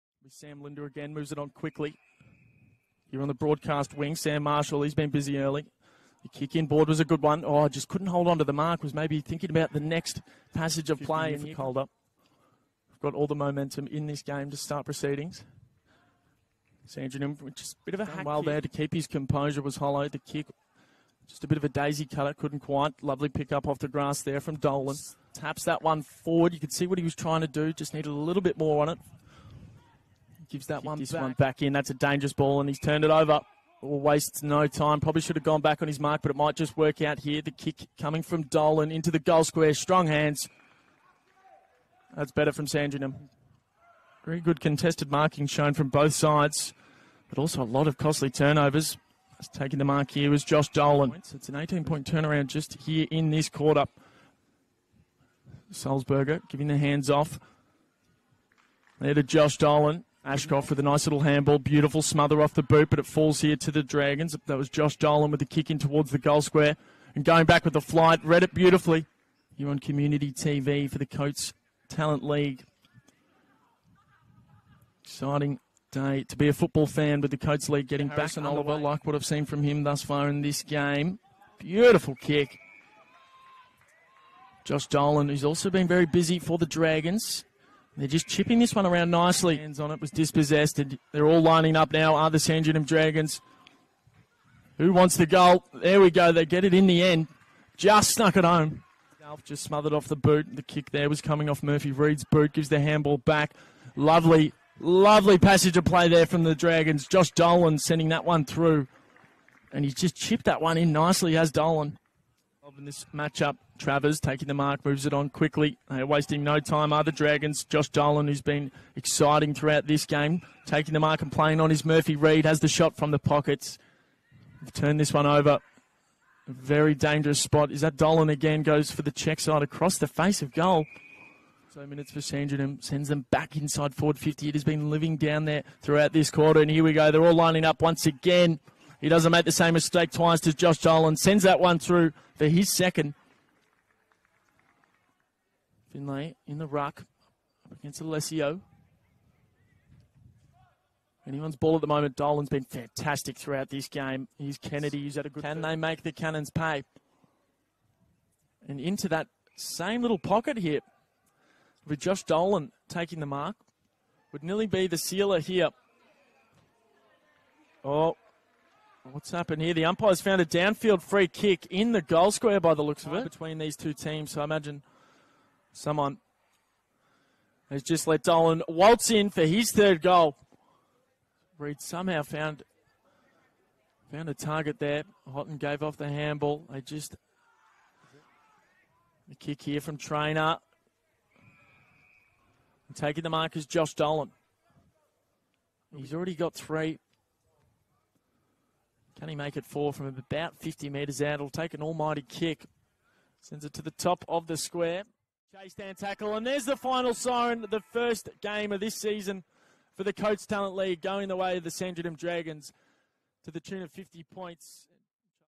Josh Dolan - Talent League Round 1
A deeper pool option, Josh Dolan is a busy high half forward who's clean at ground level, and creative and damaging with ball in hand.  He's a hard worker with excellent endurance, able to push high up the ground and get involved in transition ball movement.  He's played some through the midfield at Talent League level, but at only 177cm tall might lack the physicality for the midfield at AFL level.

If you love my content and you're feeling generous then consider buying me a coffee: